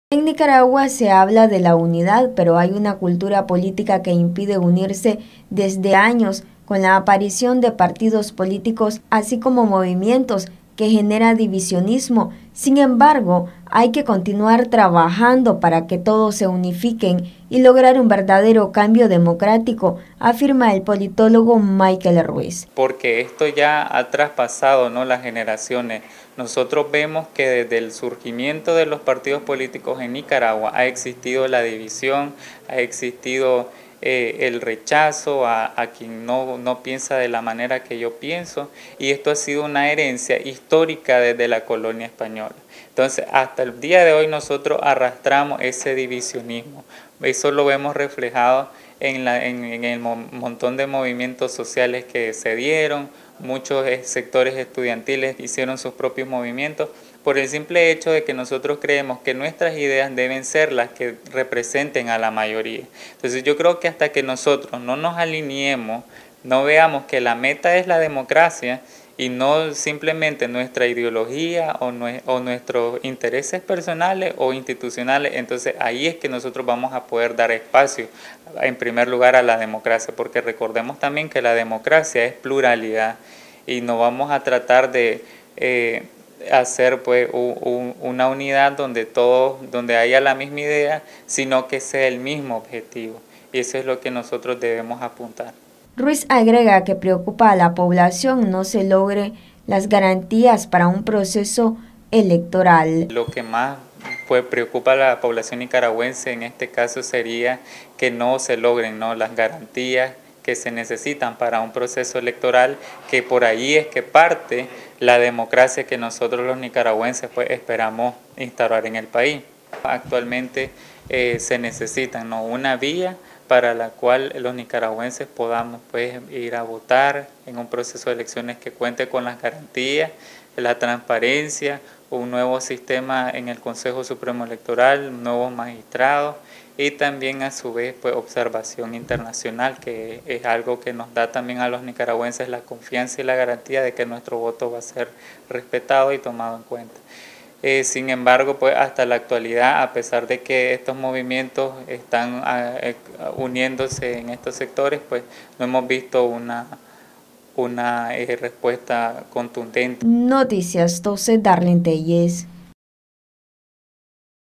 Se debe continuar trabajando por la Unidad en Nicaragua
A pesar que hay una cultura política de años que viene a impedir lograr la unidad por completo, hay que trabajar para lograrla y encontrar un cambio en la democracia afirma el politólogo Michael Ruiz.